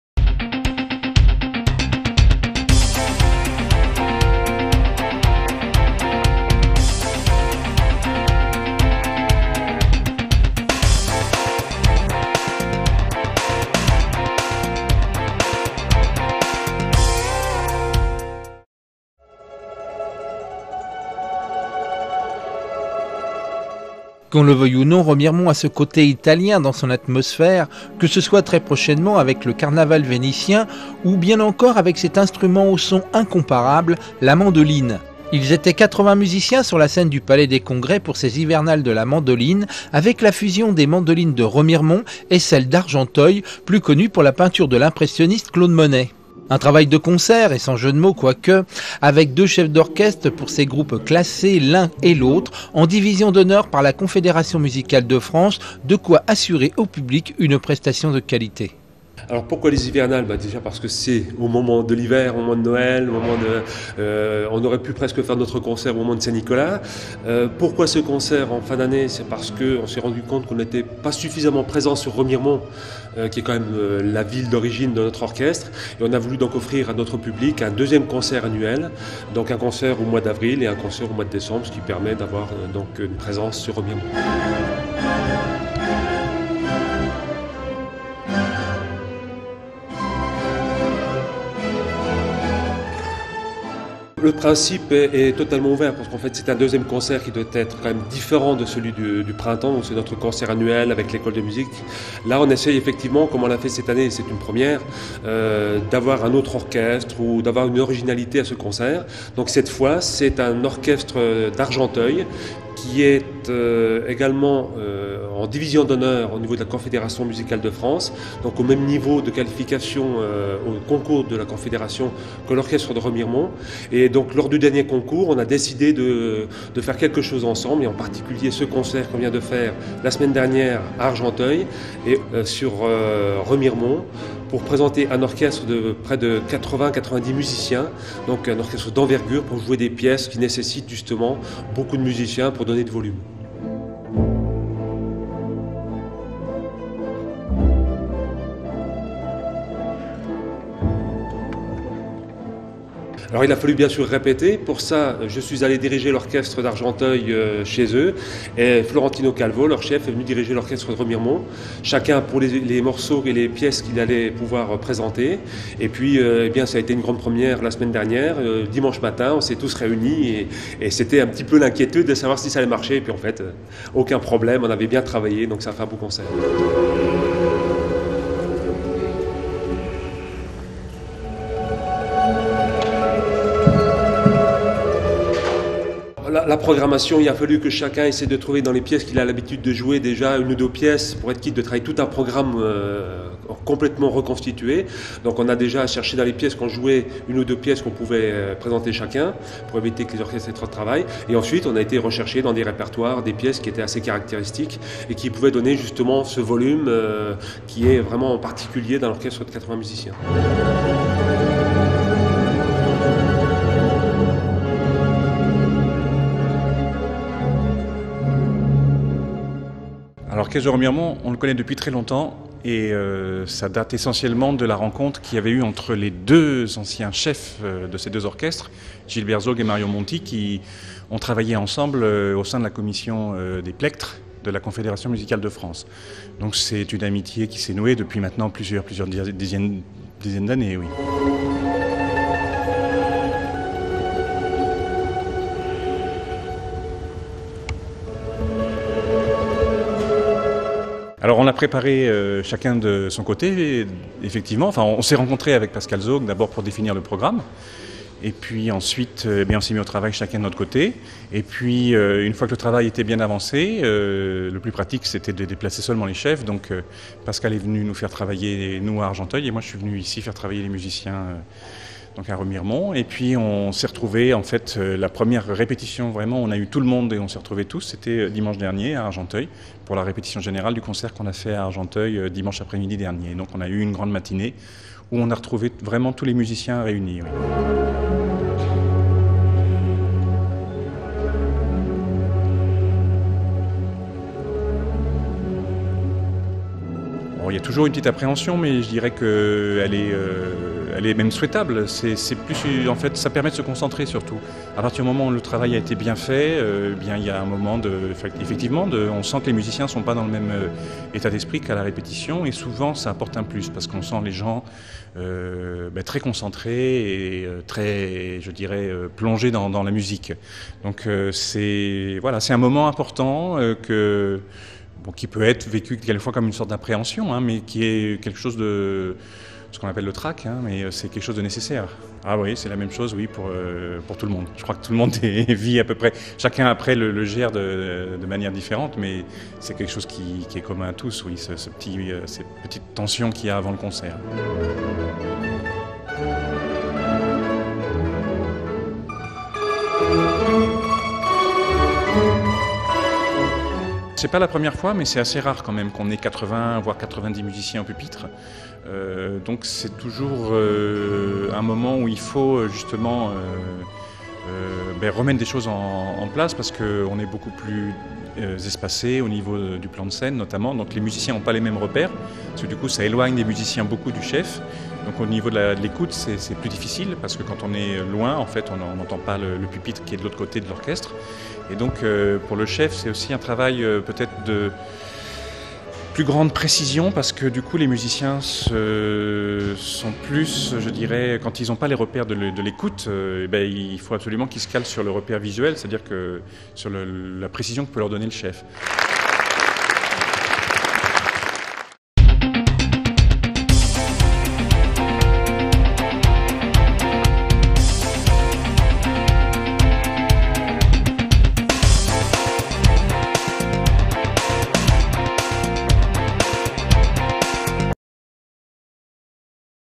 Orchestre de Mandolines de Remiremont 2013
Concert commun avec l'Estudiantina d'Argenteuil dirigé par Florentino Calvo
Reportage Vosges Loisirs Vidéo